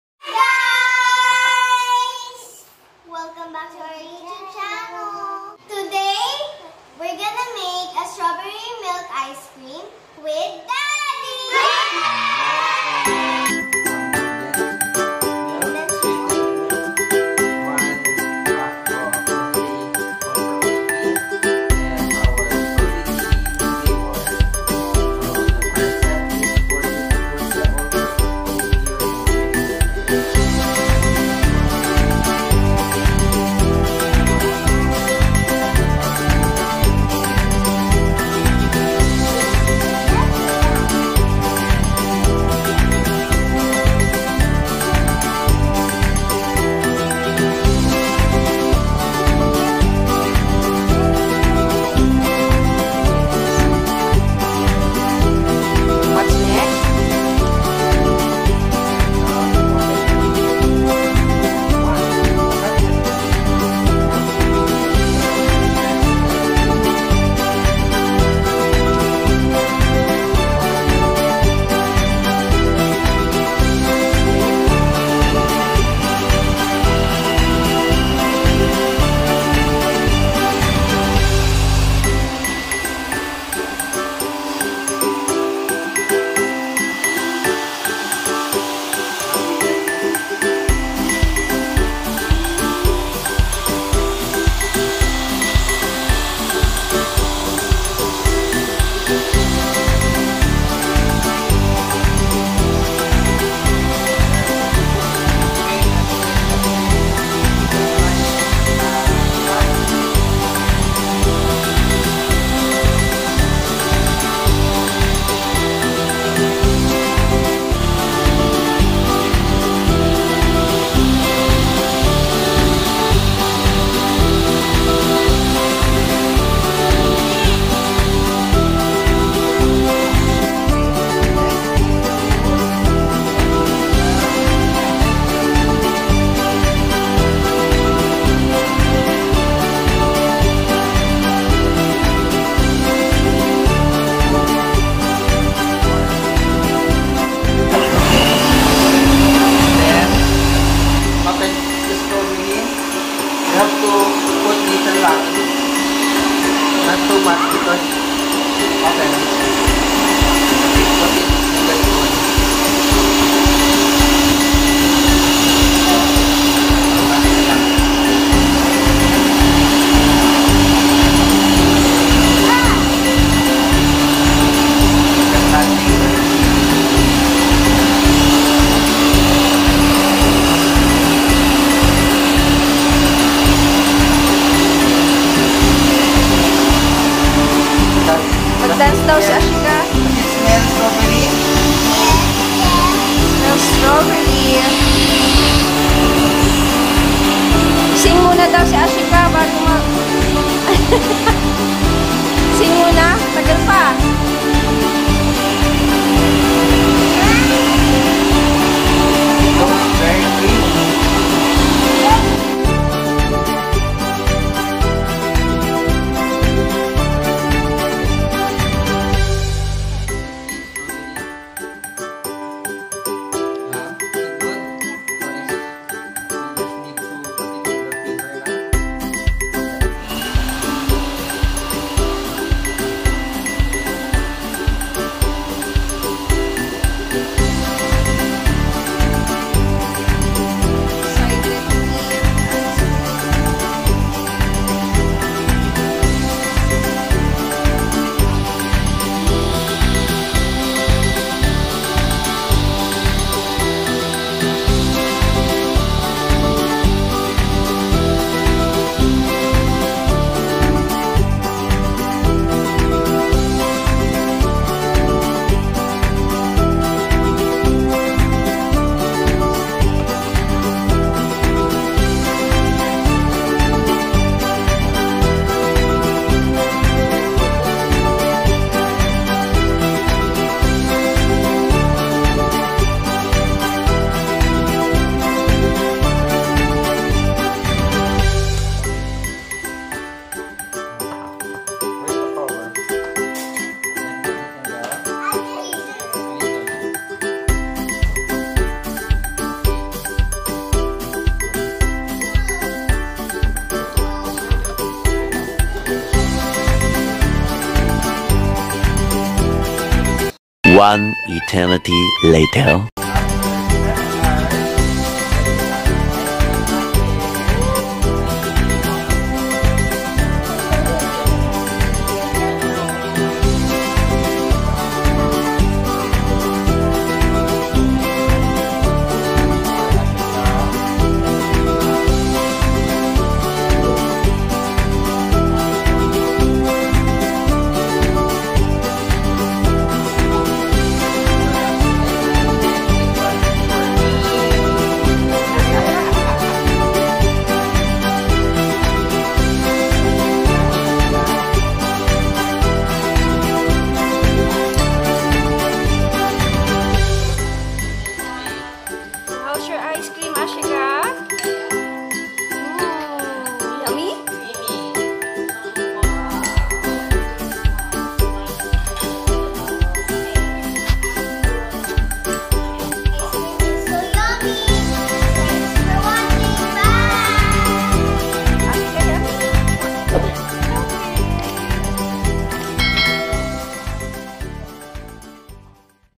Making strawberry milk ice cream with Daddy / The Untalans
Our first time to make strawberry milk ice cream.. Not bad at all😁